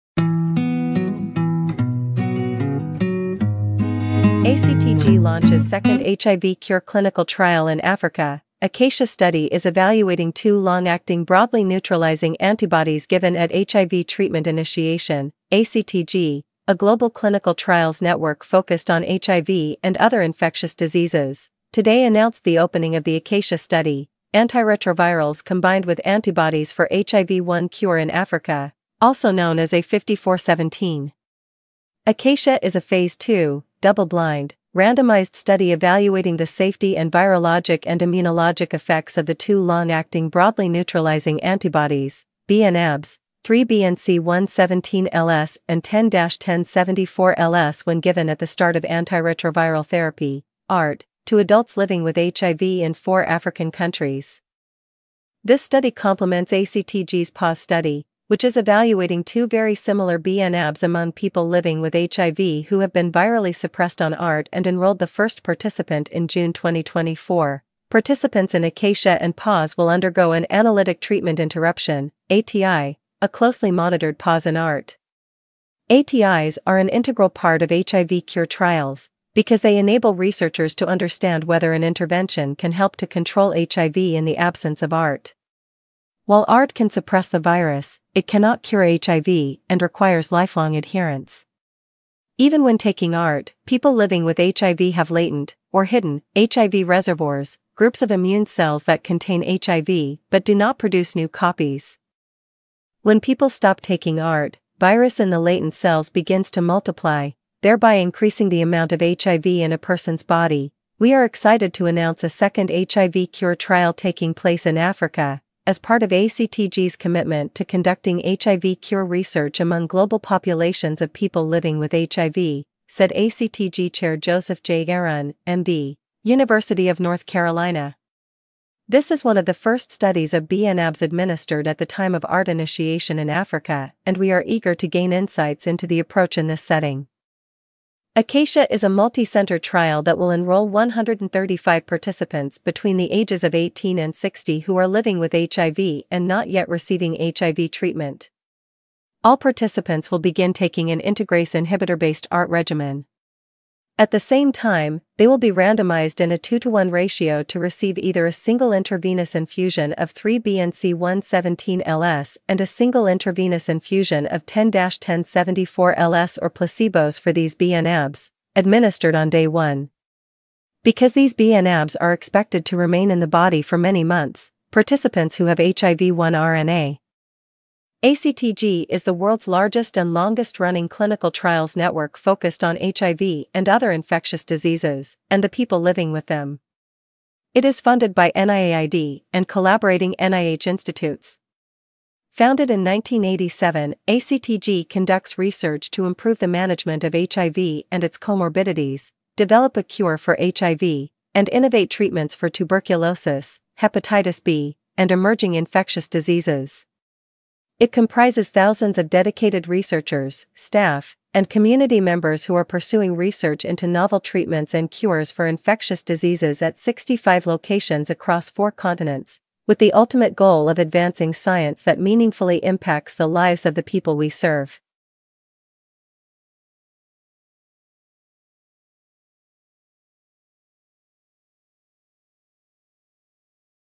ACTG Launches Second HIV Cure Clinical Trial in Africa
ACTG, a global clinical trials network focused on HIV and other infectious diseases, today announced the opening of the ACACIA study (Antiretrovirals Combined with Antibodies for HIV-1 Cure in Africa), also known as A5417. ACACIA is a phase 2, double-blind, randomized study evaluating the safety and virologic and immunologic effects of the two long-acting broadly neutralizing antibodies (bNAbs) 3BNC117-LS and 10-1074-LS when given at the start of antiretroviral therapy (ART) to adults living with HIV in four African countries.